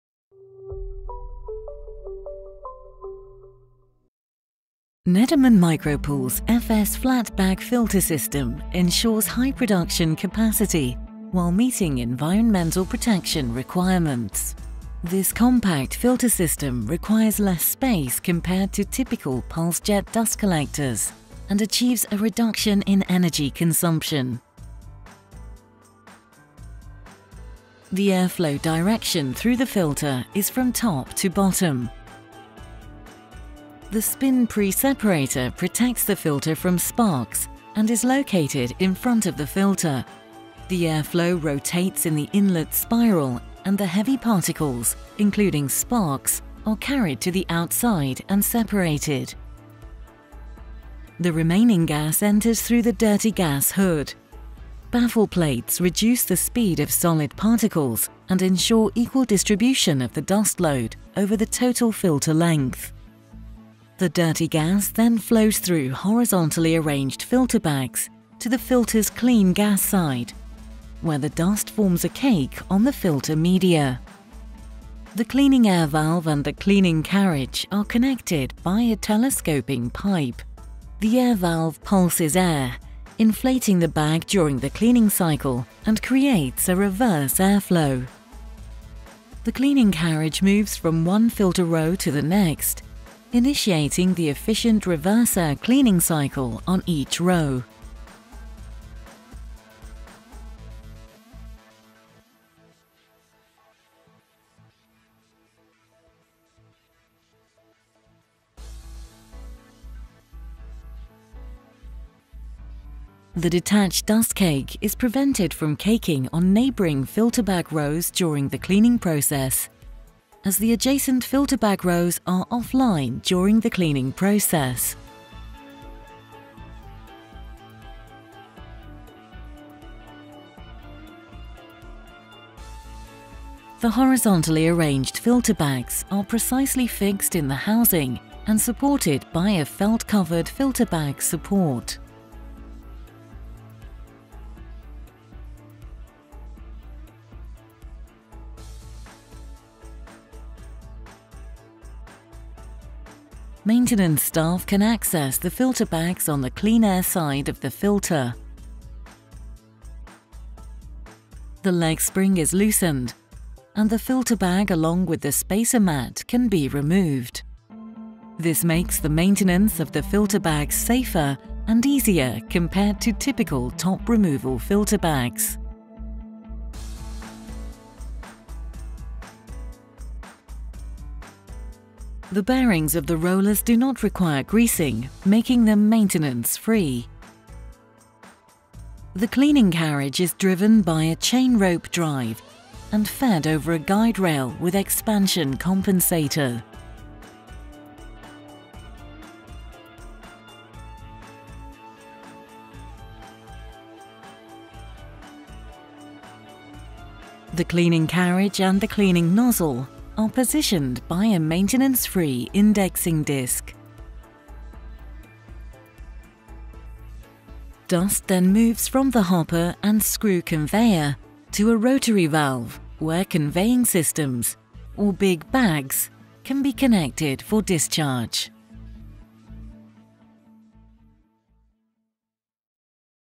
FS Dust Collector with Spin Pre-Separator
The FS series dust collector with horizontally installed flat bag filters is a compact and efficient filter system with intelligent air cleaning technology. This system is ideal for high dust loads of finer dust particles and high gas temperatures. It can be equipped with our pre-separator to help prevent sparks from reaching the dust collector and causing a dust explosion.